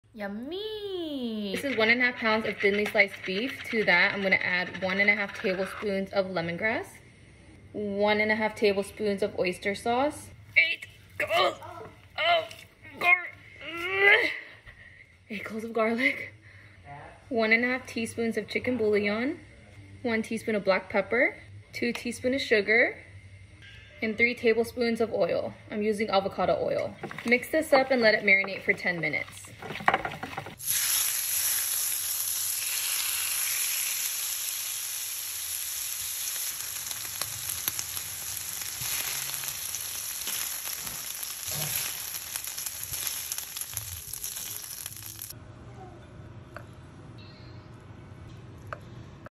Make this for dinner if you’re looking for something light &refreshing | lemongrass beef vermicelli
LEMONGRASS BEEF VERMICELLI BOWL⤵️

1.5 lbs thinly sliced beef
1.5 TBSP minced lemongrass
1.5 TBSP oyster sauce
8 cloves minced garlic
1.5 tsp chicken bouillon
1 tsp black pepper
2 tsp sugar
3 TBSP oil

1. Marinate beef with all of your ingredients for 10 mins

2. Then cook on a skillet. About 1-2 minutes each side un touched.

3. Serve with vermicelli fish sauce and veggie of choice. I used lettuce, microwaved bean sprouts, cucumbers, fried shallots, spearmint and perilla leaves